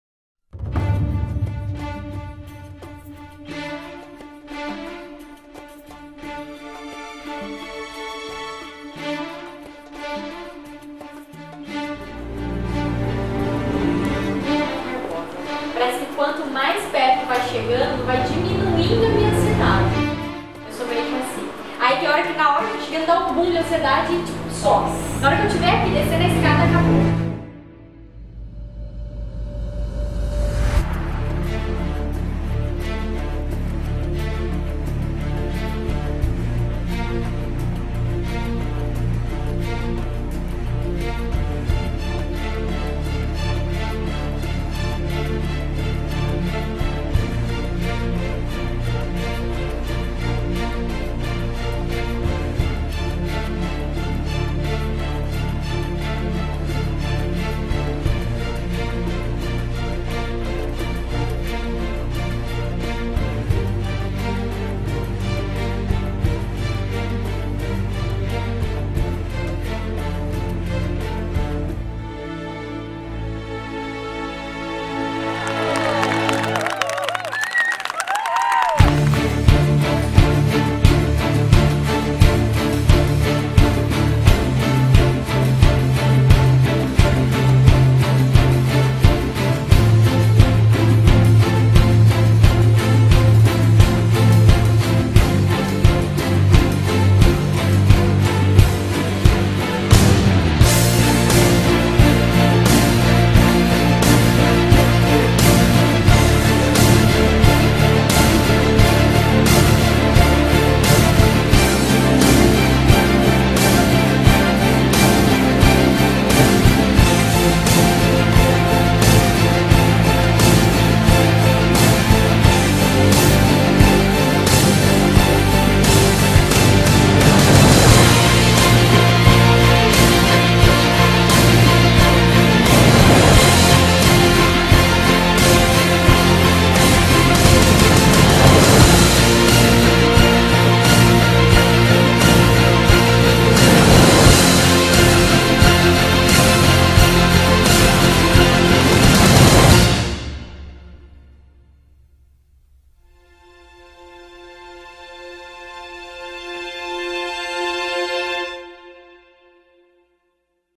Reginaldo + Gisely
Teaser - Reginaldo e Gisely.
Imagens Aéreas com Drone da praia do Massaguaçu.